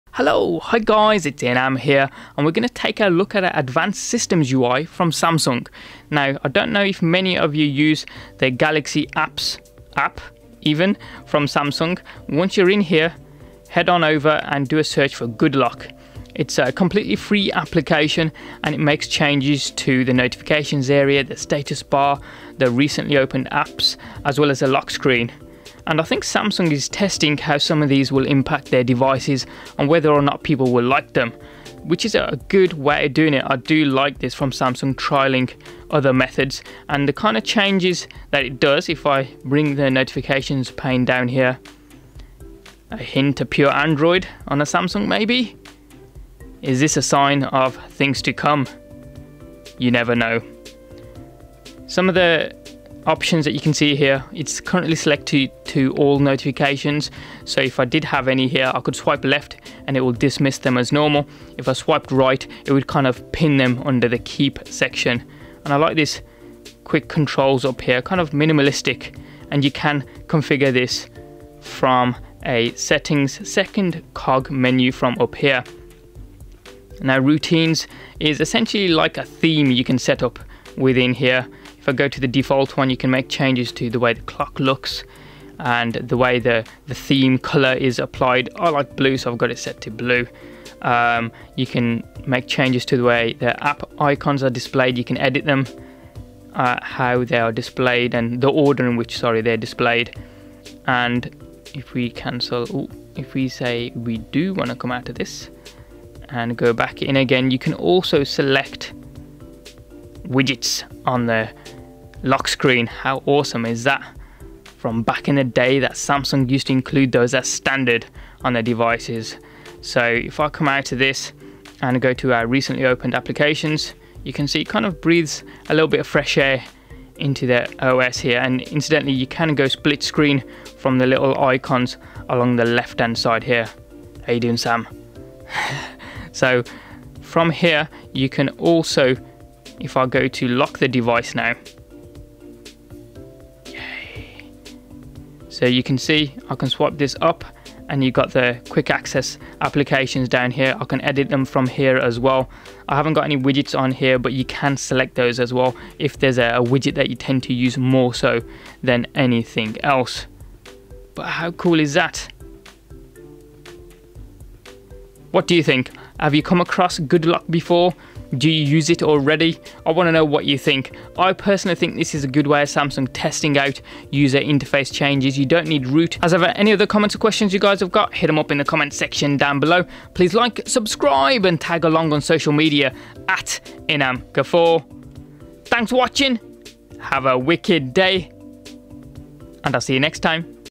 Pure Android on a Galaxy? Samsung Good Lock!
The closest to getting Pure Android on a Samsung Galaxy S7 is using the Samsung Good Lock app, have you ever heard of it?